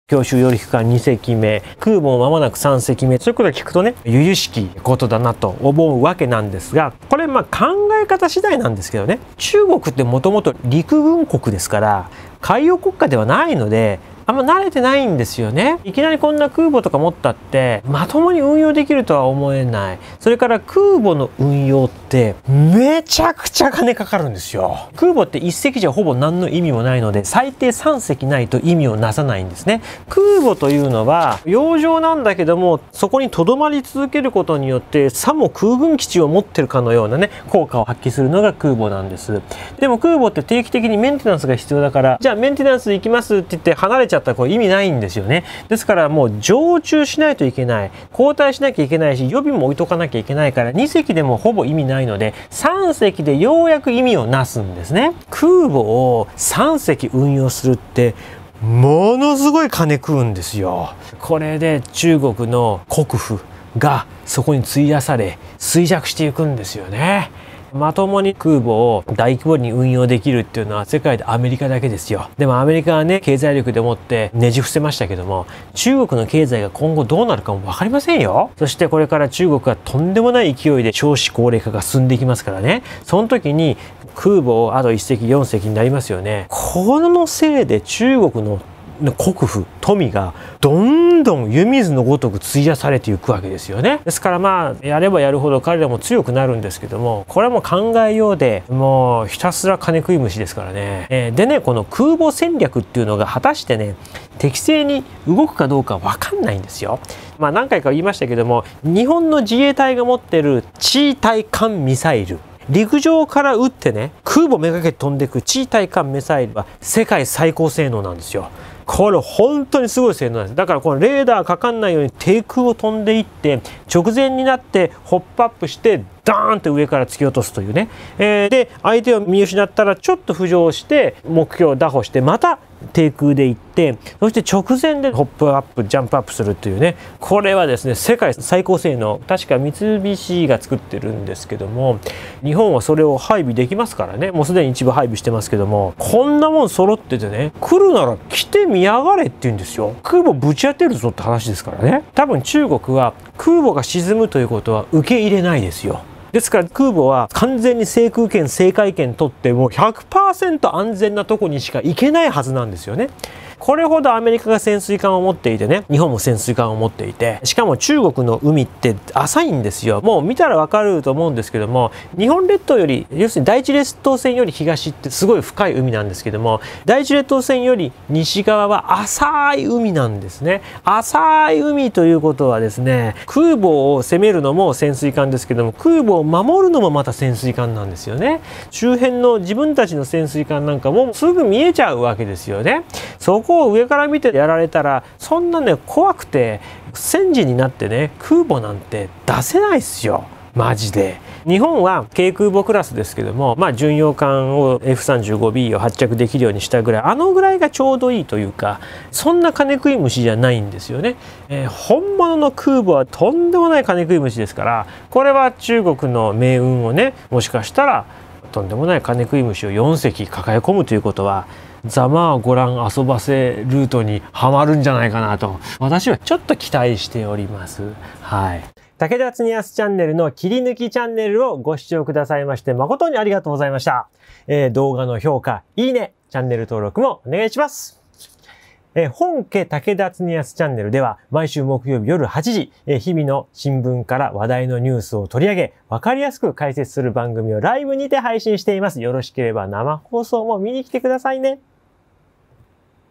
恐怖！中国の海軍増強！しかし、弱点も実はあるんです。
空母の運用は大変なんです！
陸軍国家の中国が、どこまでできるか？